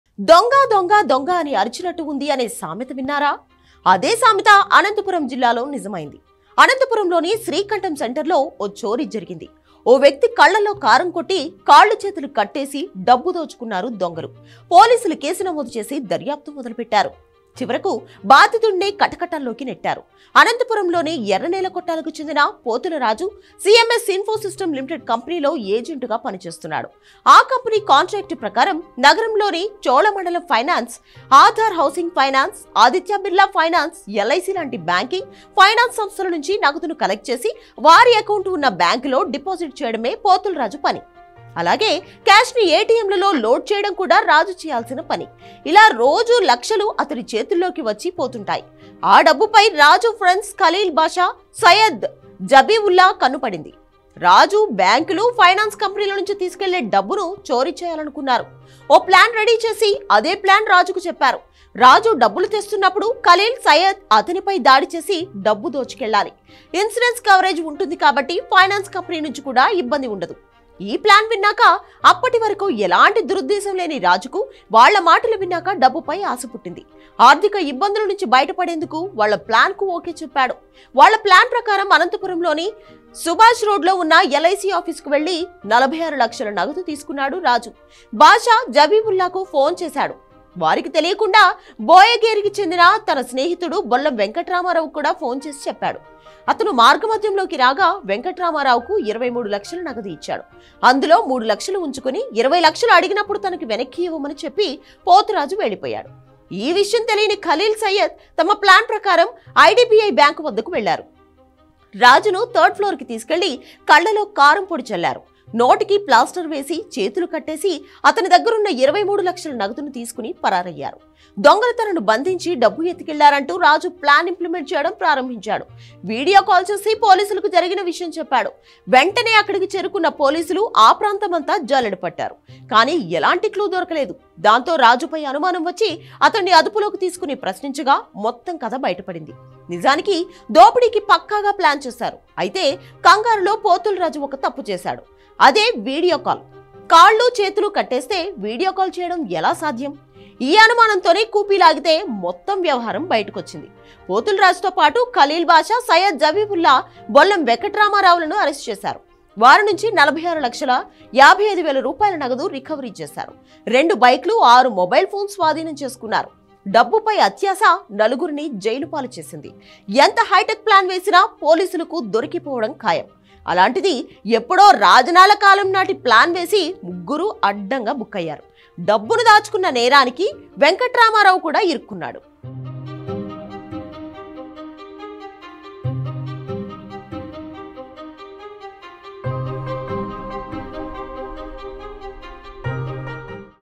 అనంతపురంలో చోరీ గ్యాంగ్‌.. వీళ్ల ప్లాన్‌ చూస్తే మతి పోవాల్సిందే || Anantapur District incident
అనంతపురంలో చోరీ గ్యాంగ్‌.. వీళ్ల ప్లాన్‌ చూస్తే మతి పోవాల్సిందే || Anantapur District incident || Dial News